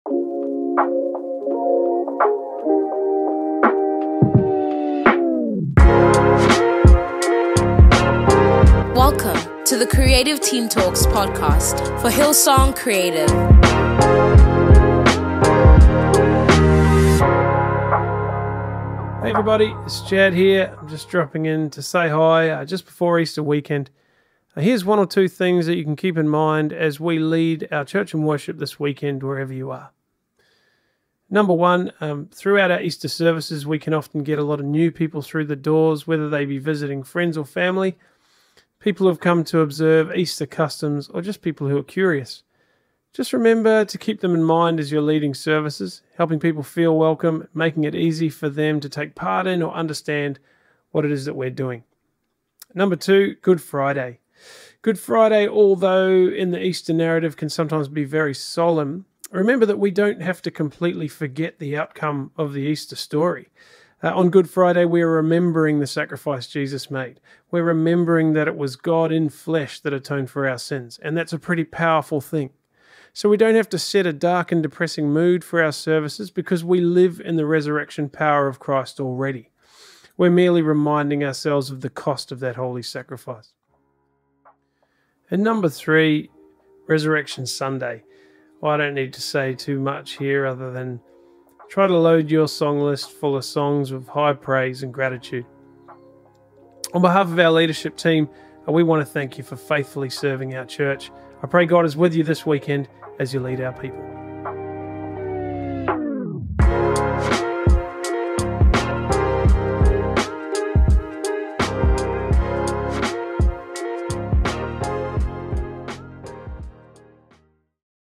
07 Three Reminders for Easter Weekend
A few quick reminders as we prepare to lead our church in worship this Easter weekend. Thank you for serving our church this Easter, wherever you are!

Created by: Jad Gillies
Produced by: JP Starra
Artwork by: Yoseph Setiawan & Kristin Mateika
Intro by: Shelby Mtsamayi